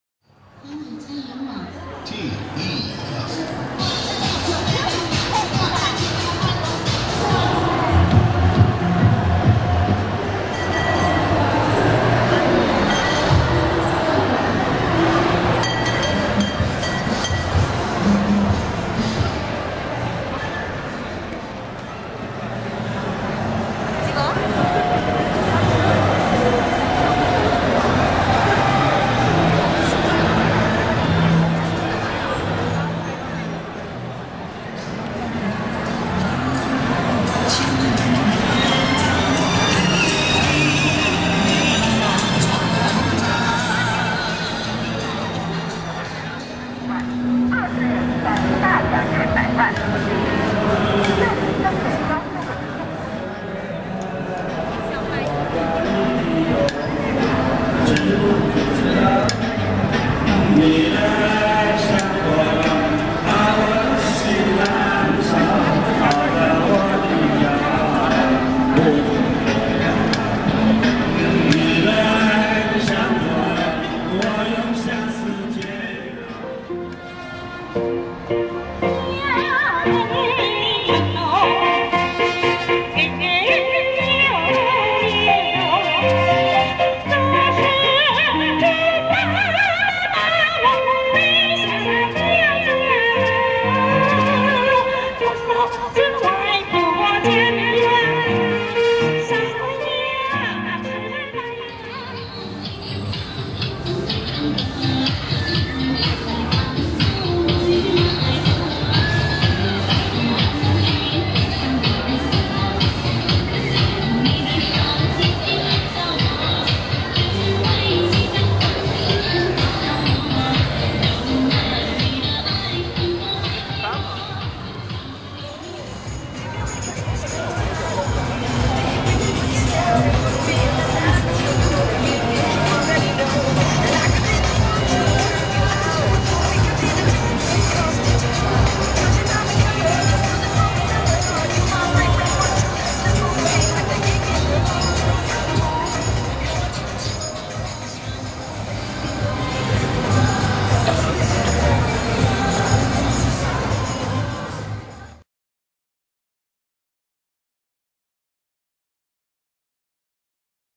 Night on Namkyung street in Shanghai 상하이 남경로의 밤풍경
Shanghai Namkyung street by night.
What a exciting place!
Check out more photo of Shanghai's night life on our blog.

LuCKi's Travel

상하이 남경로의 화려한 밤입니다.
분위기는 명동인데, 남녀노소 뛰어나와 신나게 즐기는 곳이예요.
나이에 상관없이 인생을 즐길 줄 아는 중국인들의 흥이 담긴 멋진 곳이더군요.
더 자세한 이야기는 블로그에서 확인하세요.

토종감자 수입오이의 세계여행